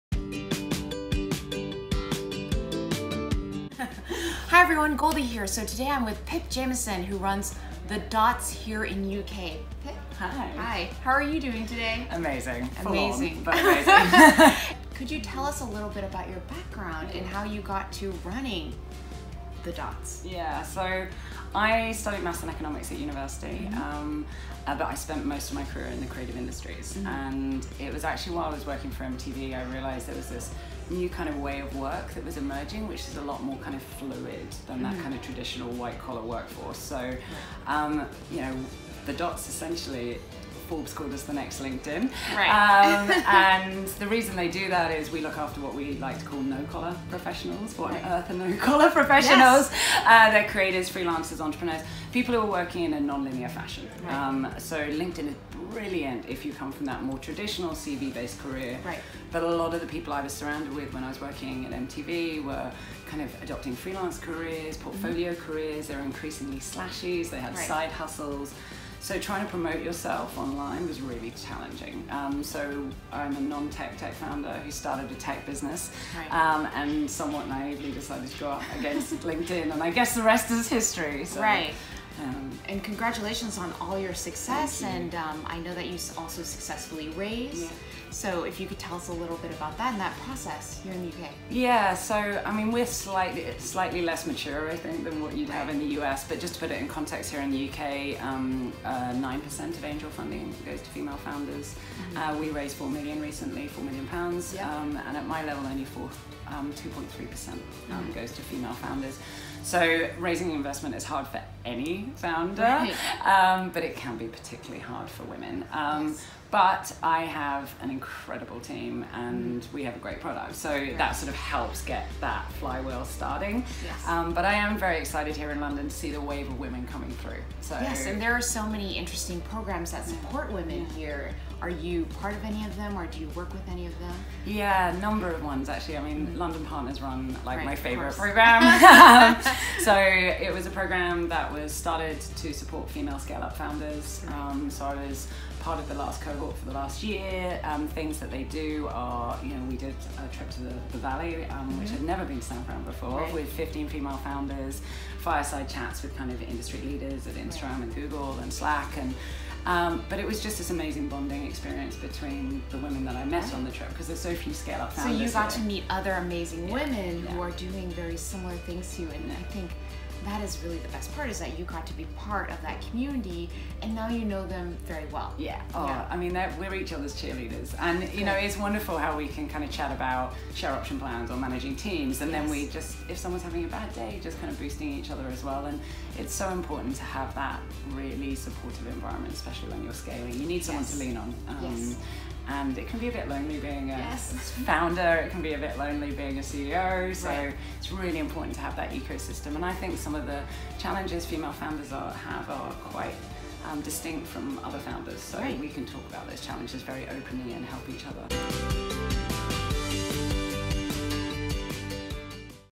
Interview with Pip Jamieson (The Dots)
Interview with CEO and Founder of Dots Global, Pip Jamieson.